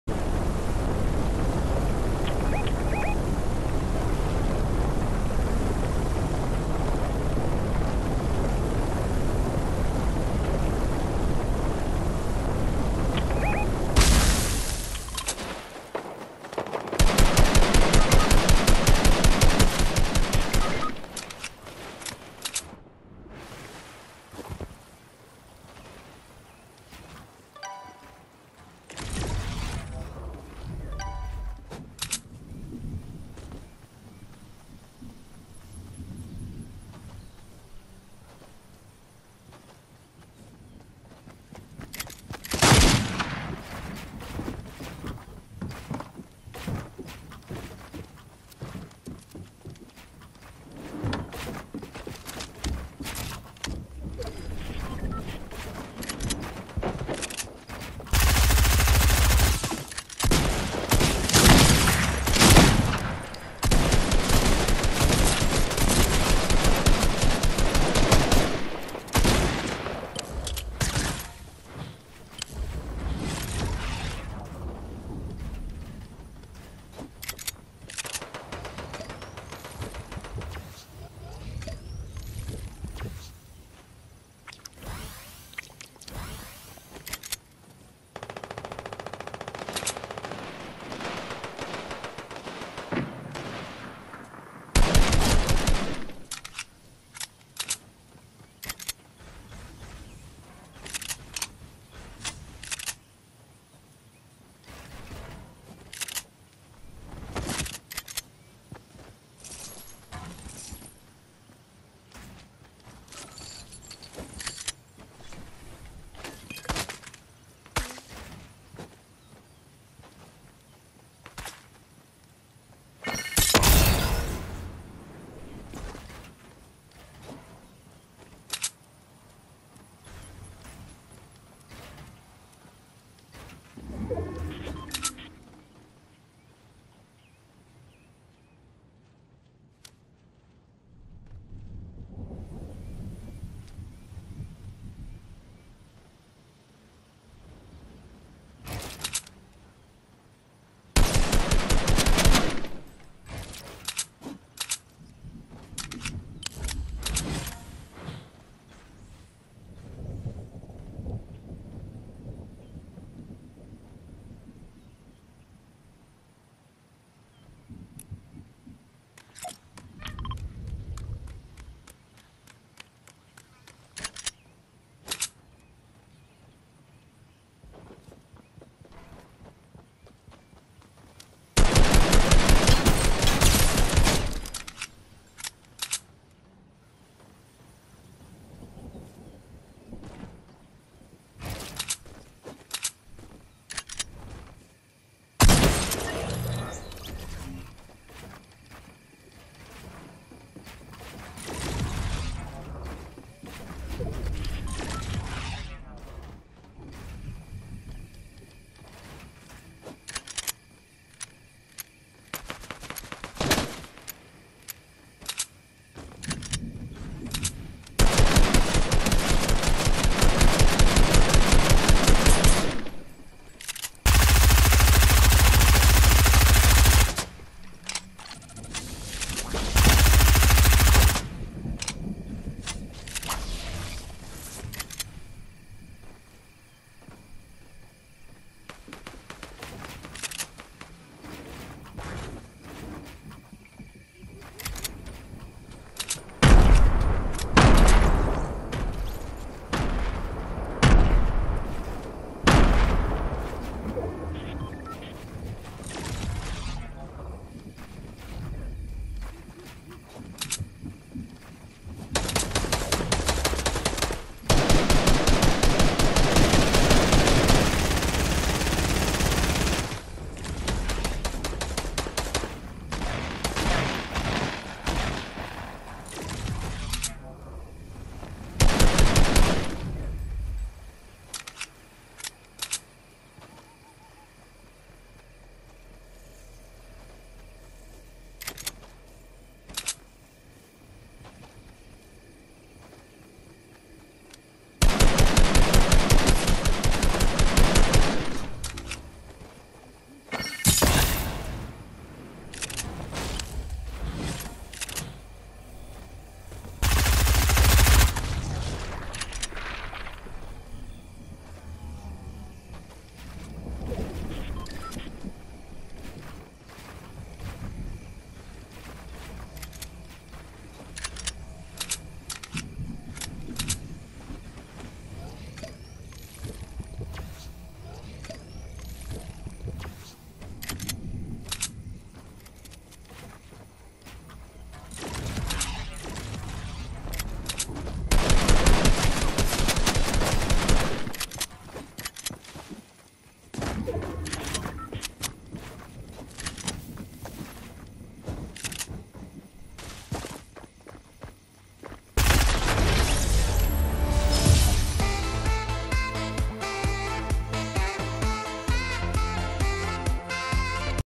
Daily victory royale 538 lone soldier
My squad leave me for the comfort of the circle on the opposite side of the map and then decide to fight a full squad by running at them with no shield in true ramboesque style only to end up catching bullets with their face and leaving me in the middle of 3 teams to fend for myself and clutch up from the safety of my tower